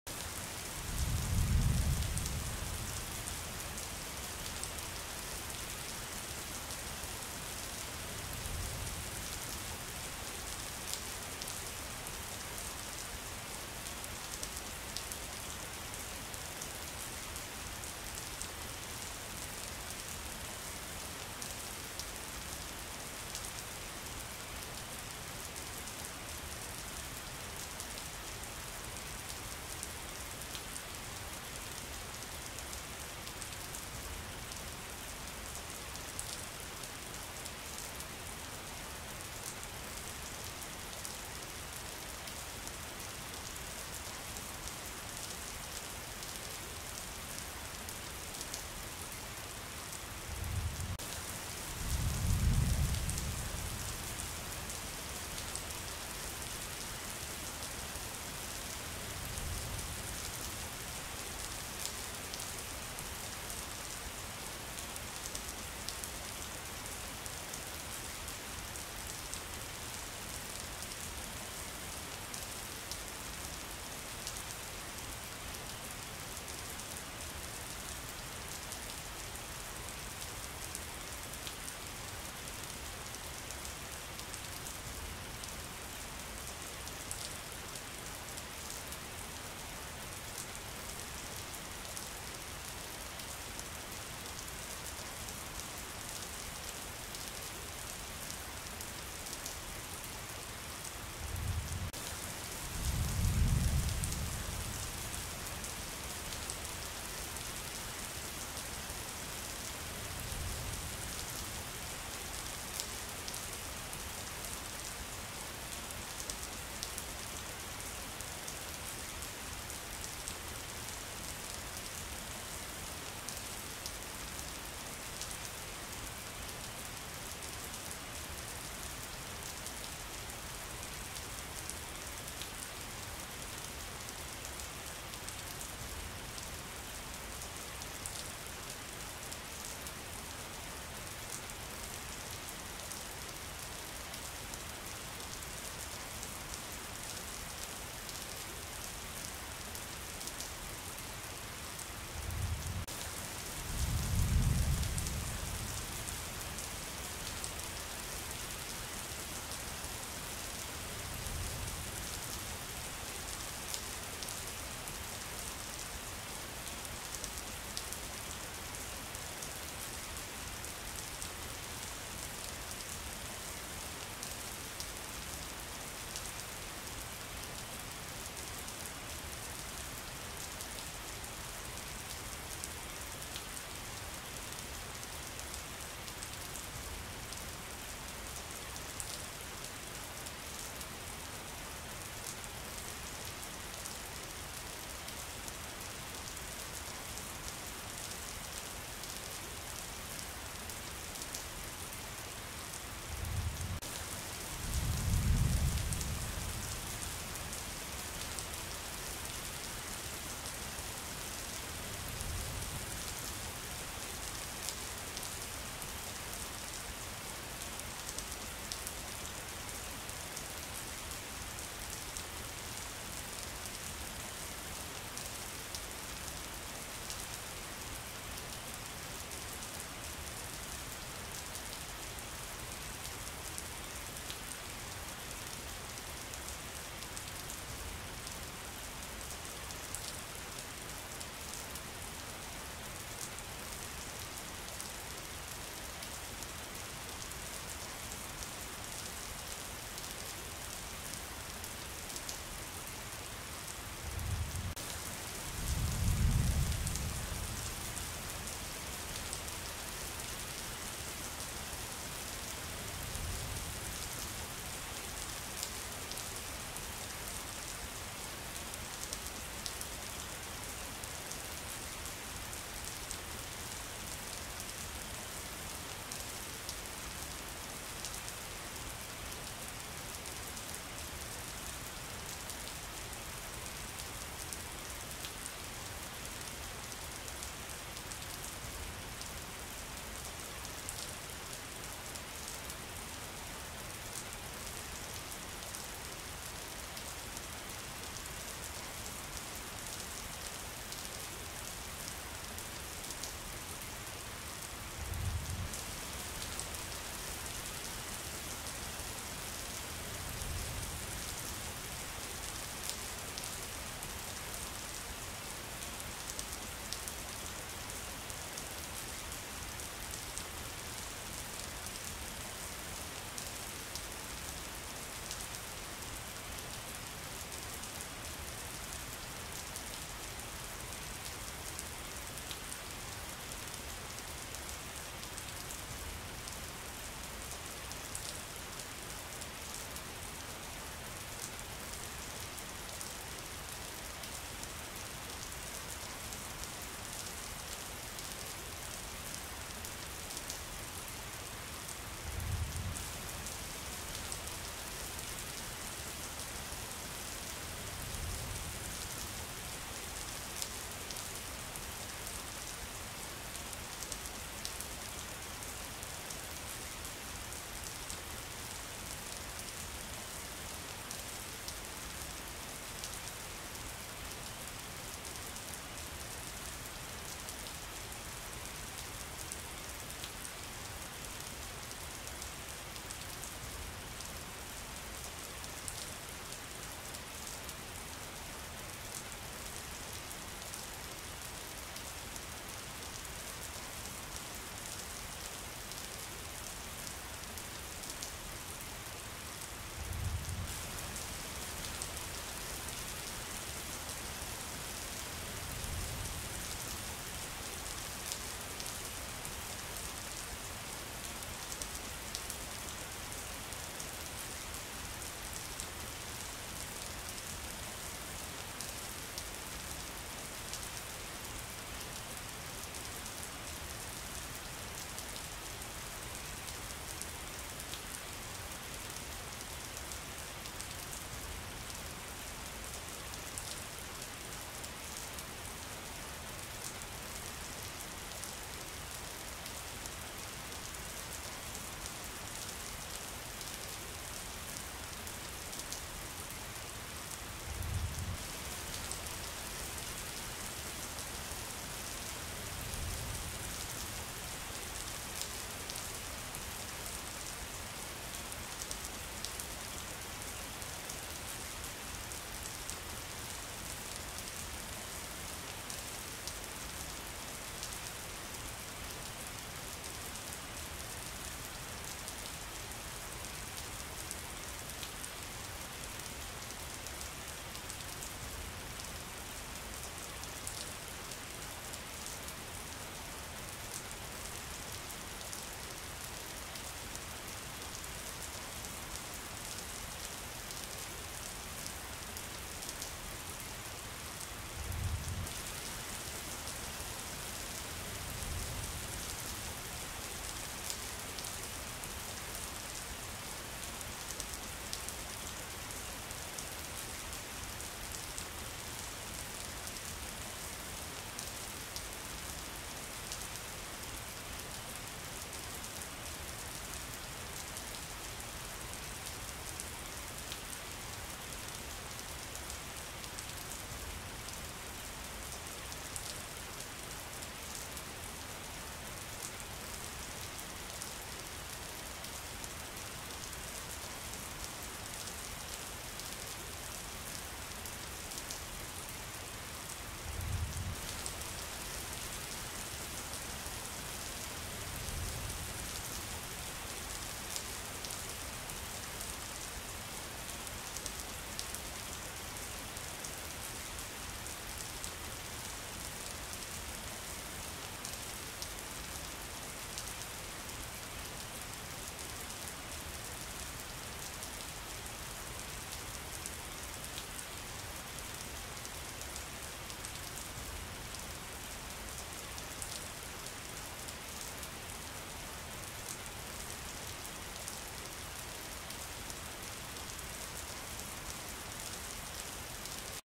Sounds of nature rain for relaxation and sleep
Relaxing rain sounds for meditation, relaxation, relaxation, sleep. Are you looking for relaxation and peace of mind? This rain is for you!
 The best relaxation - the sounds of nature are great for a short break. You can listen during your lunch break or in the evening before bed.
Atmosphere of rain, Heavy rain, Relaxing rain, Rain for sleep, Relaxing rain for the soul will help relieve fatigue and recharge your batteries!
Happy listening and have a good rest!